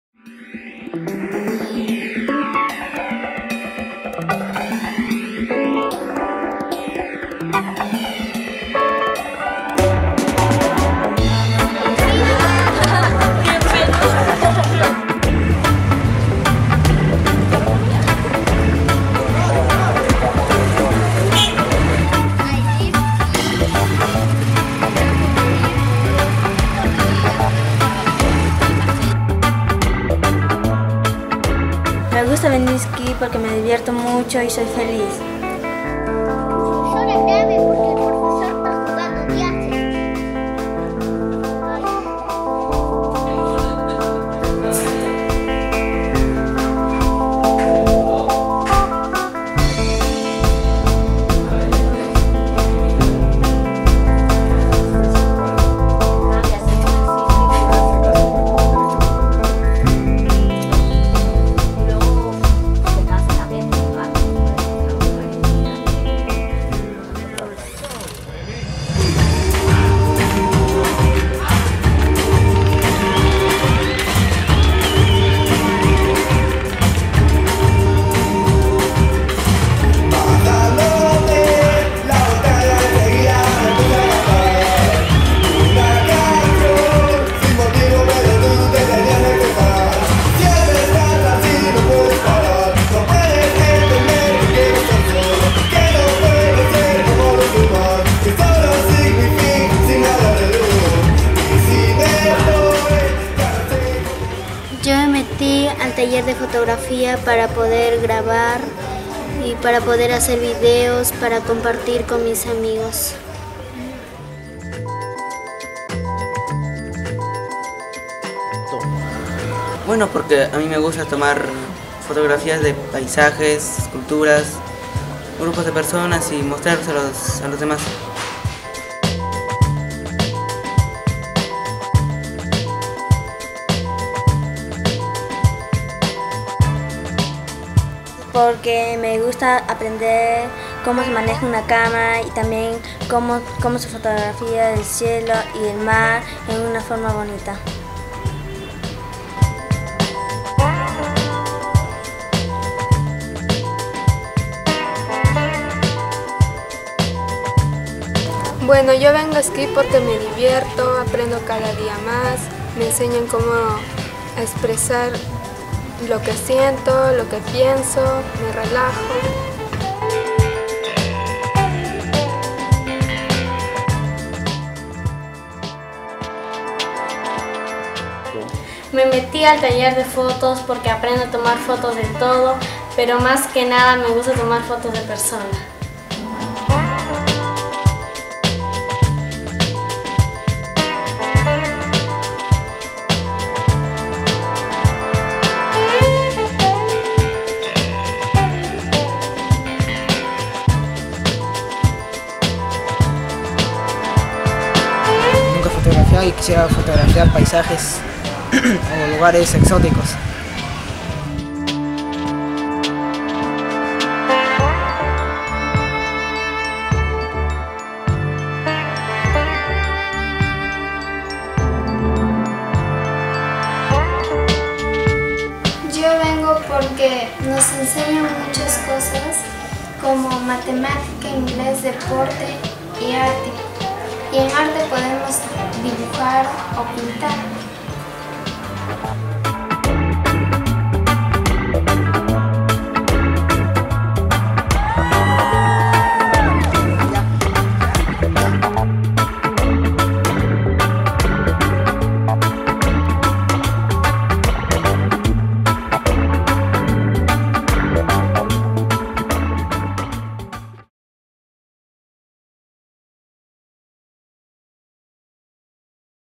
Young People Tell us What they Love About SKIP - ¿Por qué me encanta SKIP?
This wonderful video has been made by some brilliant and creative volunteers and young people from the SKIP programme. This forum provides a great opportunity for the young people to talk about what they enjoy about SKIP and, in particular, the photography workshops we run each year.
The short film will be shown at our upcoming fundraiser and exhibition in New York on April 10th in Brooklyn ReBar DUMBO 7-10pm.

Este video increíble ha sido un proyecto de un grupo de voluntarios y jóvenes maravillosos. Es una oportunidad para los jóvenes a hablar sobre lo que es divertido en SKIP, especialmente las talleres de fotografía que hacemos cada año.